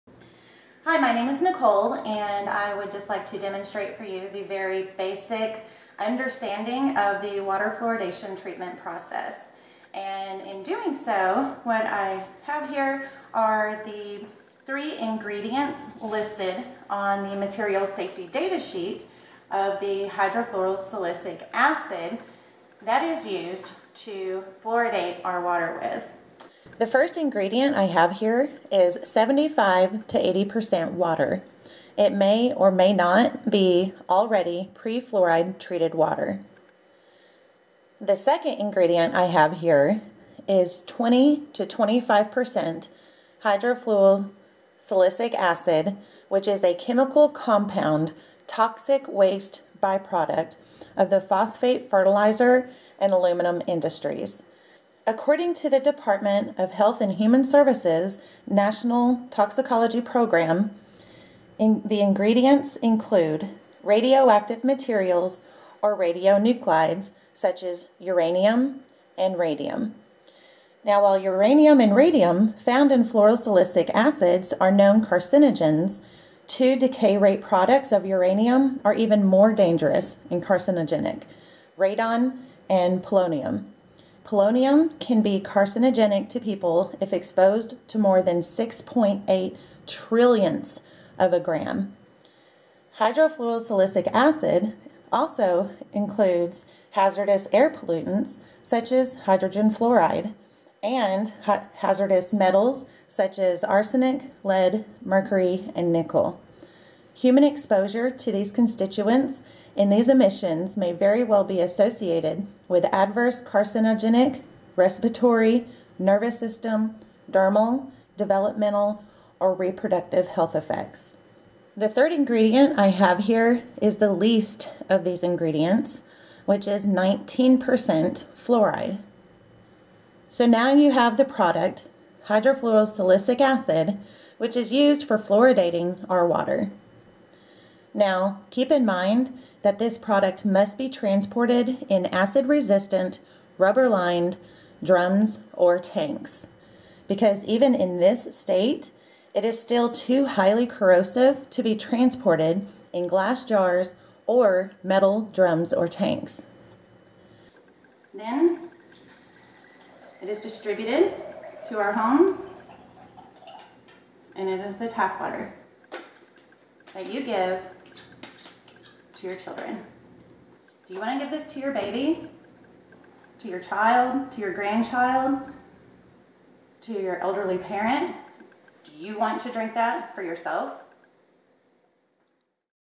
VERY Basic Understanding of Water Fluoridation Treatment Process
STOP CALLING IT FLUORIDE!
Our water is 'fluoridated' with a product called Hydrofluosilicic Acid which is a chemical compound toxic waste by-product of the phosphate fertilizer and aluminum industries.
The actual product is made up of 75-80% Water, 20-25% Hydrofluosilicic Acid, 19% Fluoride.

INGREDIENTS & USES
Hydrofluosilicic Acid contains Radioactive materials or Radionuclides such as: Uranium and Radium. While uranium and radium found in fluorosilicic acids are known carcinogens, two decay rate products of uranium are even more dangerous and carcinogenic: Radon-222 and Polonium-210  (Polonium-210 can be carcinogenic to people if exposed to more than 6.8 trillionths of a gram)

It also includes Hazardous Air Pollutants such as hydrogen fluoride; and hazardous metals such as arsenic, beryllium, cadmium, chromium, manganese, mercury, and nickel. Human exposure to these constituents in these emissions may be associated with adverse carcinogenic, respiratory, nervous system, dermal, developmental, and/or reproductive health effects.

Besides being used to 'Fluoridate' water supplies, this product is used in the tanning of animal hides and skins, in ceramics and glass etching, in technical paints, in oil-well acidizing, in the manufacture of hydrogen fluoride, for the sterilization of equipment (like brewing and bottling establishments and for copper and brass vehicles), and in electroplating. It is also employed as an impregnating ingredient to preserve wood and harden masonry and for the removal of mold as well as rust and stain in textiles.

HFA is a moderately strong acid that can corrode glass and stoneware. Like its salt, its degree of hydrolysis is essentially 100% in drinking water, and when reacted with steam or water or when heated to decomposition or highly acidified, toxic and corrosive fumes of fluorides (e.g., hydrogen fluoride and silicon tetrafluoride) are released. It also reacts with metals, producing hydrogen gas.

According to the Department of Health and Human Services -- National Toxicology Program's Literature of Fluorosilicic Acid
The manufacture of phosphate fertilizer releases not only fluorides as a toxic pollutant, but also radionuclides. Radium wastes come from the filtration systems. Uranium and its decay-rate products are found in the phosphate rock and fertilizer as well as the byproduct - fluorosilicic acid. During the wet-process procedure, both radium and uranium are captured in the scrubbers and therefore are in the fluorosilicic acid.
And then, during the acidulation process, radon gas can be released and carried into the fluorosilicic acid, while polonium can be captured and then combine with fluoride.

The HHS - National Toxicology Program Literature also states:
The main routes of entry of fluorosilicic acid are inhalation and eye & skin contact.